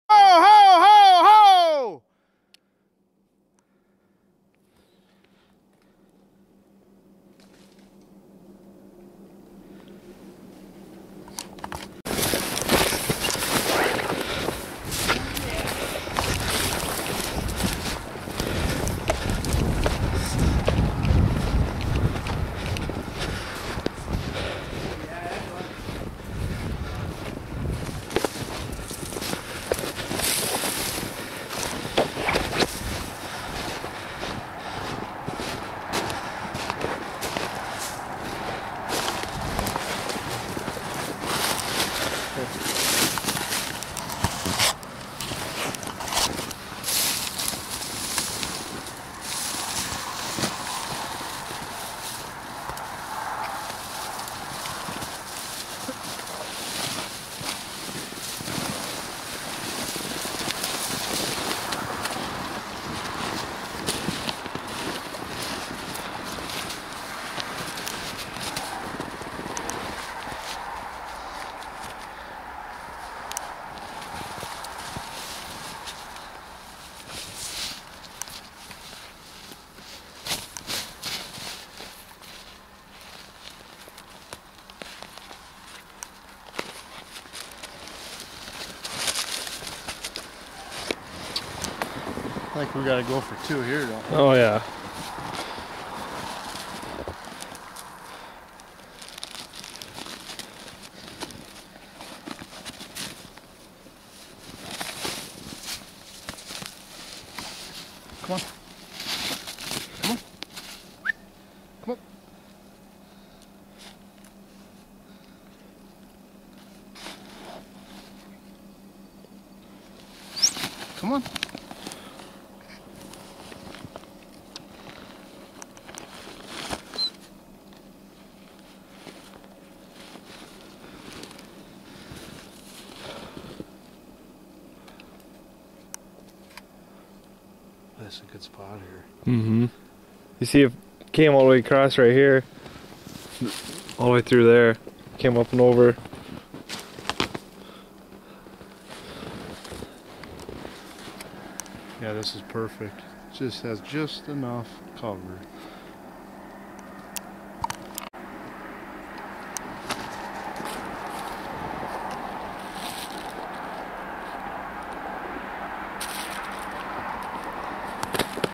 falconry,redtail hawk #5
rabbit hunting with my hen redtail